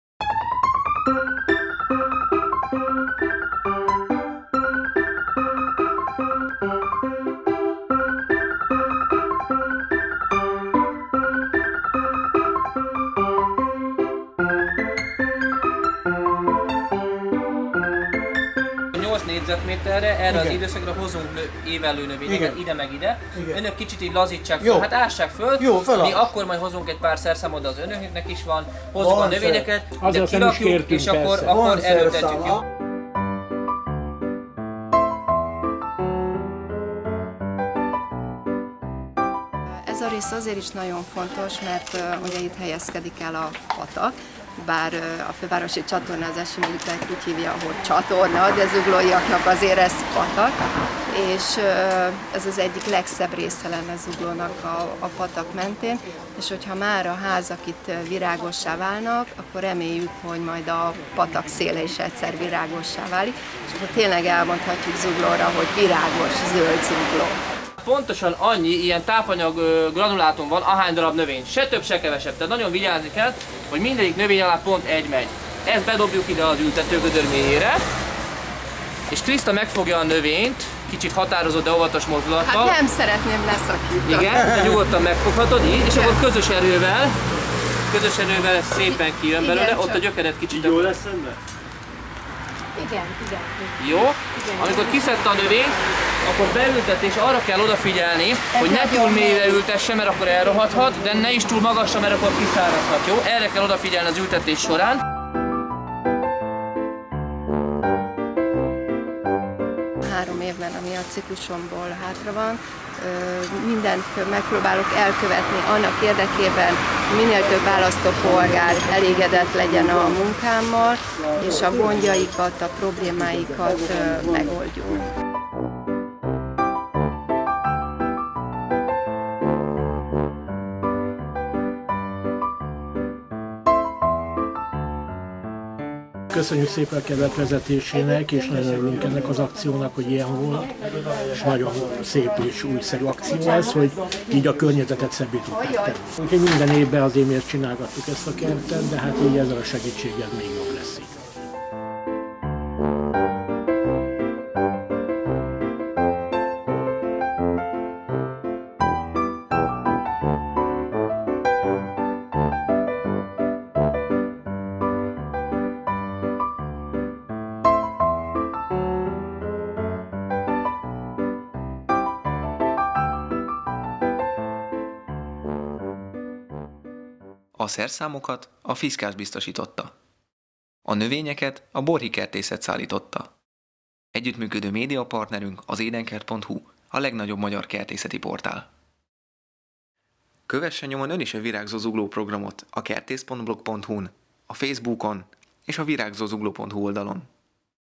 Rákospatak utca 60/d - Virágzó Zugló 2011 Program
Virágzó Zugló 2011 Utcafront szépítési program

Kövesse a Virágzó Zugló programját a - n!

Budapest Főváros XIV. kerület Zugló Önkormányzat által kezdeményezett Virágzó Zugló 2011 Program példaértékű sajátossága, hogy Zugló Önkormányzata és a zuglói lakosok közösen hozzák rendbe, teszik virágzóvá a lakóházukhoz tartozó utcafrontot, előkertet. A programban résztvevő lakosoknak az Önkormányzat térítésmentesen biztosítja az elültetendő cserjéket és virágpalántákat, valamint az ültetéshez szükséges felszerelést. Az ültetést valamint az elkészült terület további gondozását a lakóközösségek végzik.
A program szakmai koordinátora: Megyeri Szabolcs kertészmérnök.

A programban a szerszámokat a FISKARS Brands Finland Oy Magyarországi Fióktelepe biztosítja.

A program során elültetett növényeket és felhasznált kiegészítőket a Borhy Kertészet szállítja.

Egy program, amellyel mindenki nyer!

A zuglói lakók és lakóközösségek részére meghirdetett pályázat sikerességét mi sem bizonyítja jobban, mint a felhívás közzétételét követően a polgármesteri kabinethez beérkező több száz jelentkezés és az eddig elkészült, Zugló számos pontján megtekinthető több mint száz utcafronti virágágyás.

A program egész évben tart, a késő őszi fagyok beköszöntéig, tehát mindenki sorra fog kerülni, a jelentkezést követően azonban minden pályázónak türelemmel kell várnia a kapcsolatfelvételéig.
A program első szakaszában -- a helyszíni felmérést követően, előre egyeztetett időpontban -- a lakóközösség a koordinátor szakmai irányításával közösen ültetik el az önkormányzat által biztosított növényeket. Az ültetésben több alkalommal a területért felelős önkormányzati képviselők is közreműködnek.  A növények telepítéséhez szükséges kézi szerszámokat a zuglói kertbarátok részére a program támogatója, a FISKARS Magyarország biztosítja.
A második szakaszban, előreláthatóan az őszi hónapokban a szakember előzetes egyeztetést követően ismét felkeresi az elkészült utcafrontok, előkertek tulajdonosait a tapasztalatok közös értékeléséhez.
A program egyik legfontosabb célja, hogy felhívja a nyilvánosság figyelmét közterületeink rendbetételének, tisztántartásának fontosságára, ezért az ültetés folyamatáról és az elkészült területről fényképfelvétel és kisfilm is készül, ami felkerül a program honlapjára és a résztvevők is kapnak saját példányt.

Felemelő érzés az a példaértékű összefogás a lakóközösségekben, mely a főváros szívében, Zuglóban bontakozik ki a közös cél, a virágzó Zugló megteremtéséért.